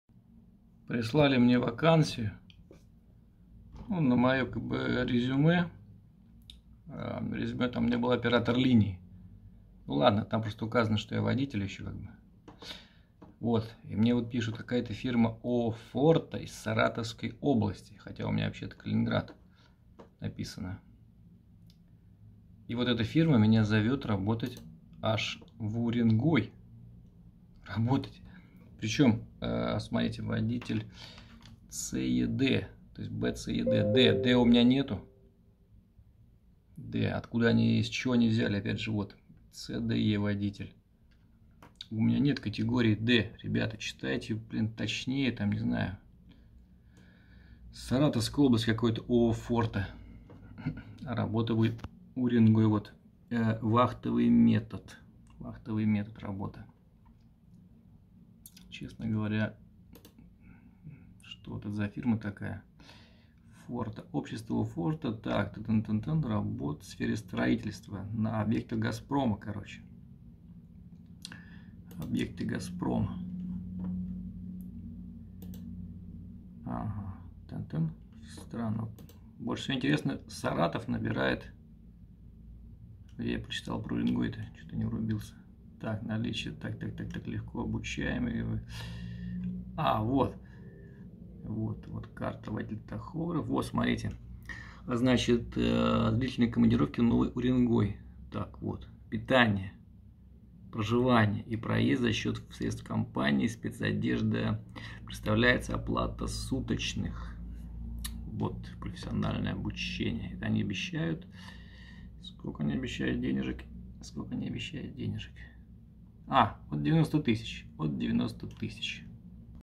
Вакансия от 90 тысяч в Новый Уренгой
Саратовские эйчары в глаза долбятся , предлагая мне вакансию с категорией D. У меня нет категории D ! Читайте резюме внимательнее, маленькие мазафакеры !